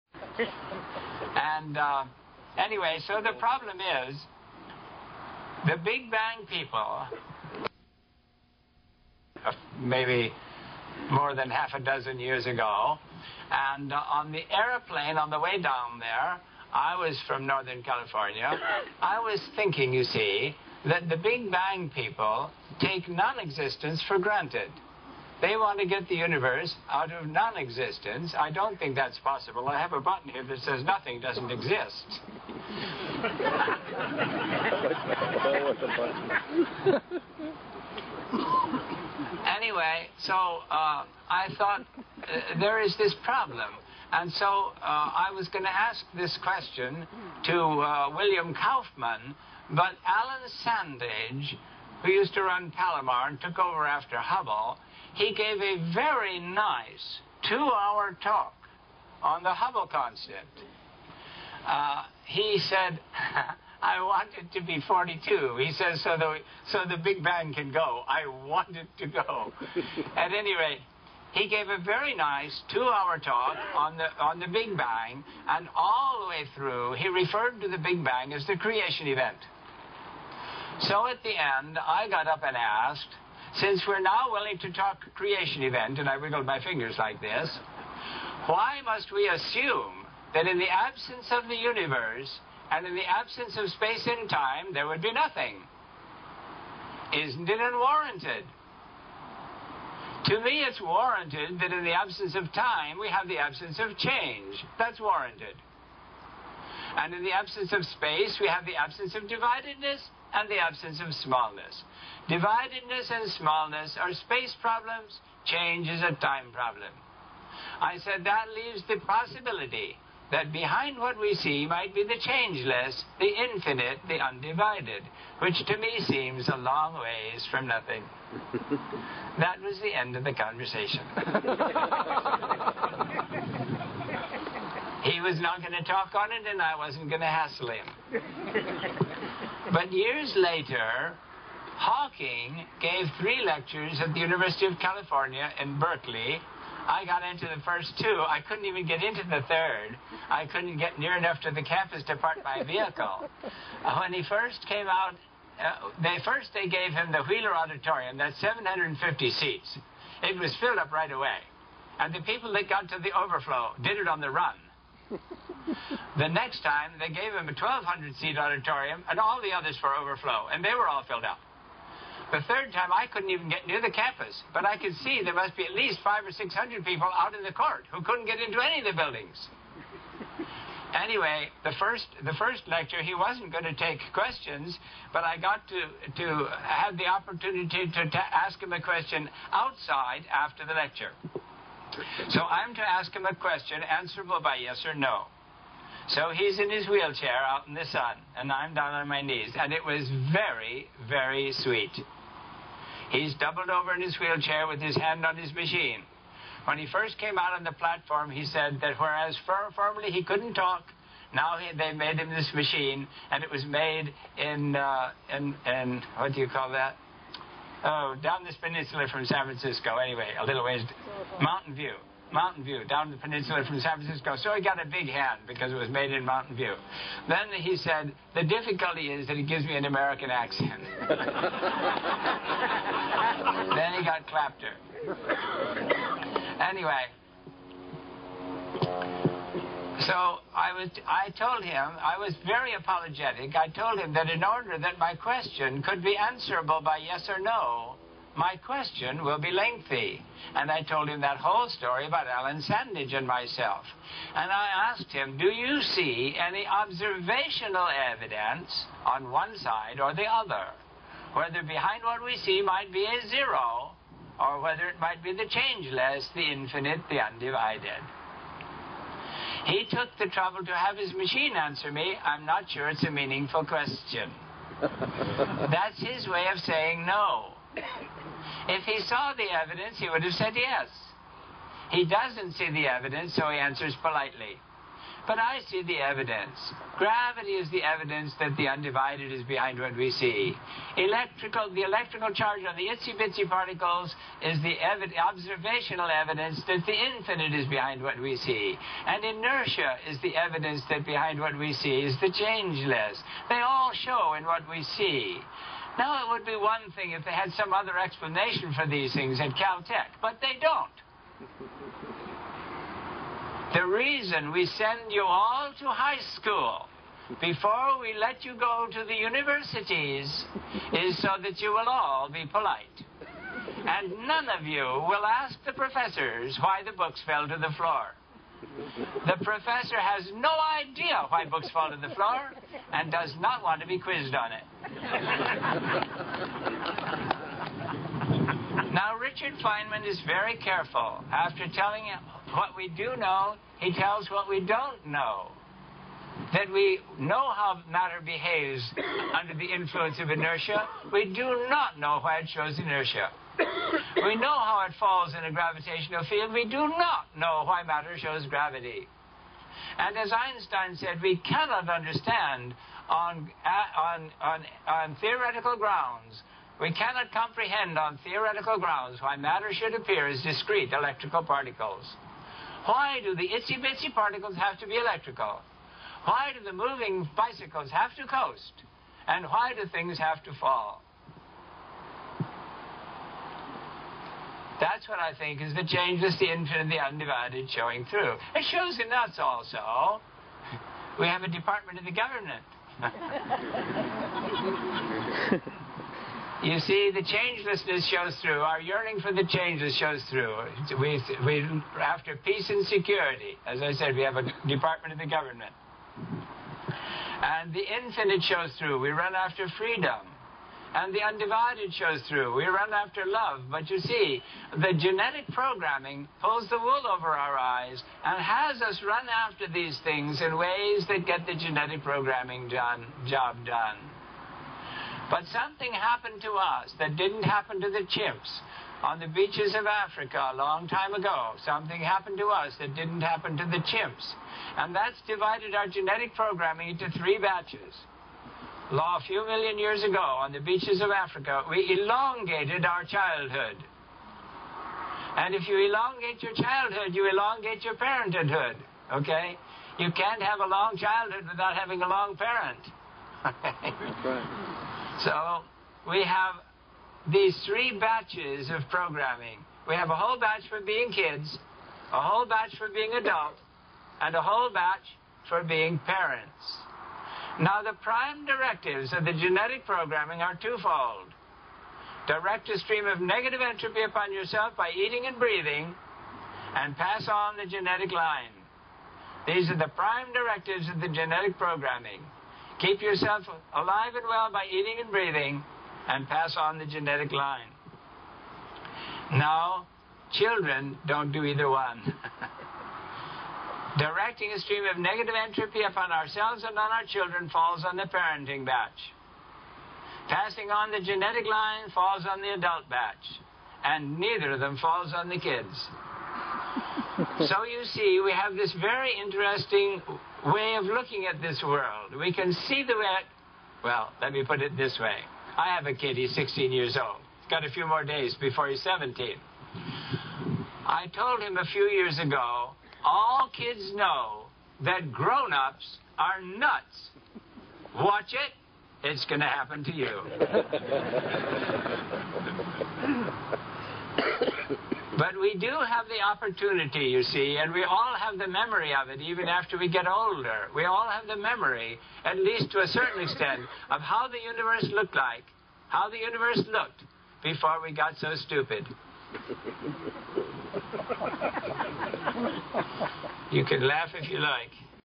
Winter Star Party 1996 with John Dobson - part 4
John Dobson of "Dobsonian Telescope" fame tells us about his past, and gives advice and opinion about amateur astronomy, science and the universe.